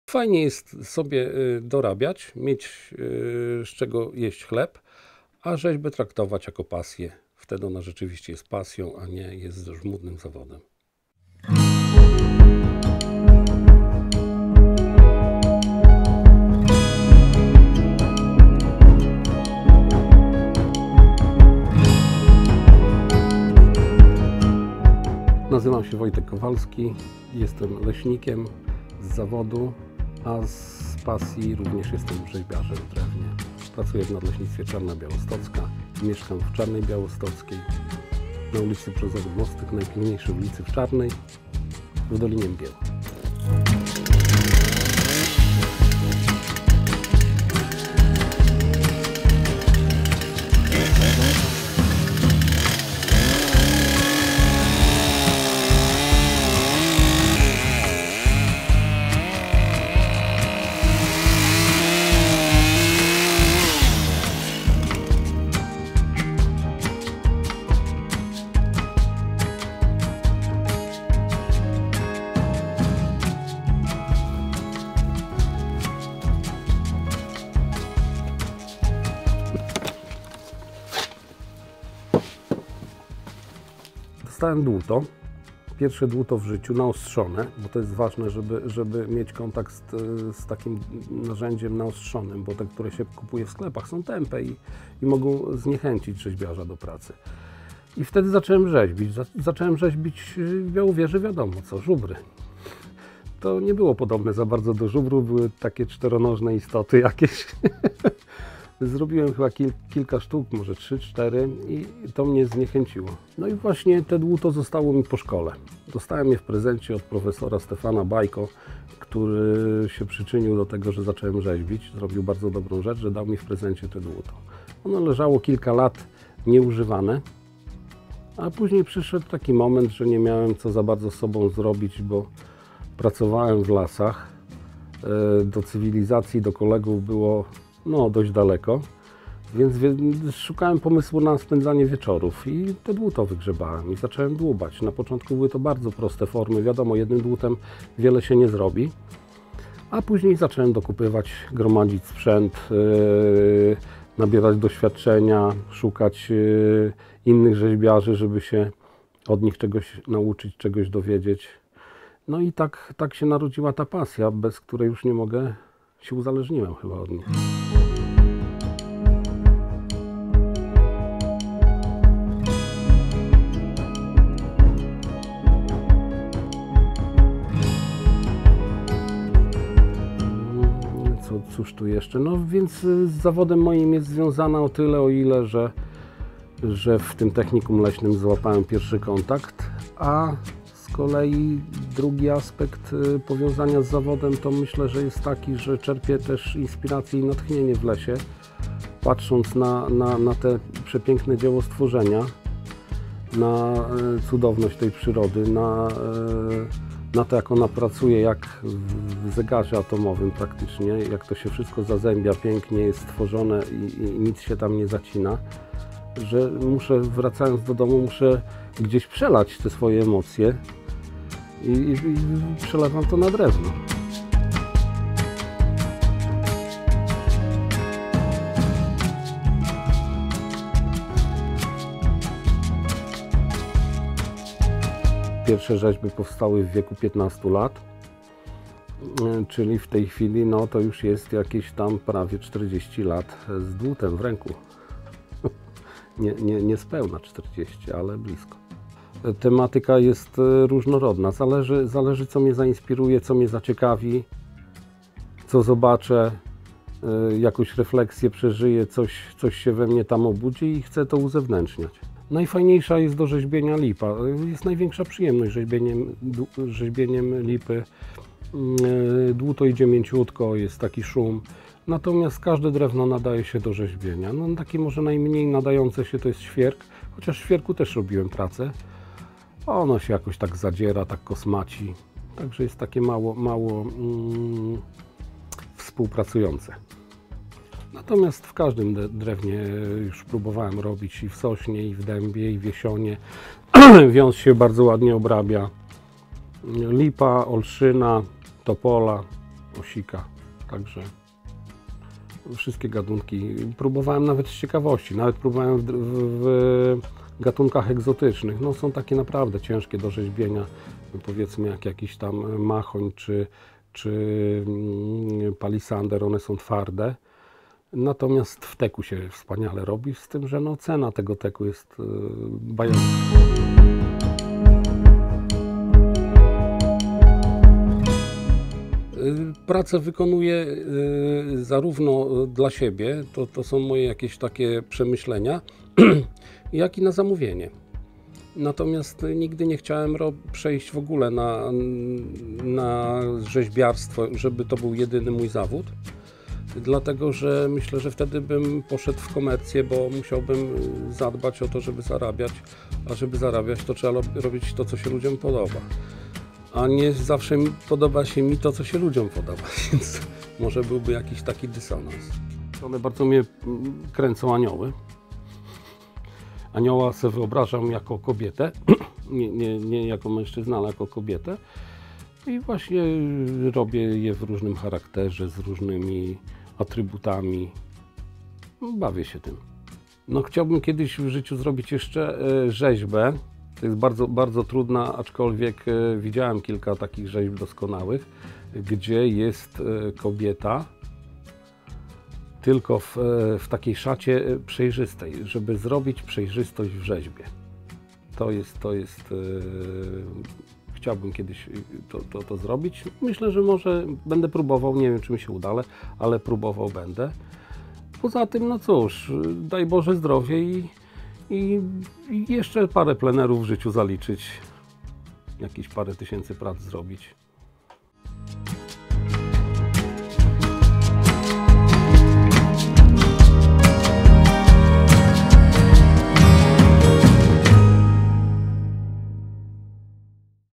Wojciech Kowalski - rzeźbiarz z Czarnej Białostockiej
Wojciech Kowalski z Czarnej Białostockiej opowiada o tym jak narodziła się jego pasja do rzeźbiarstwa. Niezwykle charyzmatyczna i ciekawa postać z naszych podlaskich terenów. Serdecznie zachęcamy do obejrzenia całości i zapoznania się z twórczością pana Wojtka.

Zdjęcia i Montaż: Adrian Ławreszuk
Rozmawiała: Natalia Wenclik-Olszewska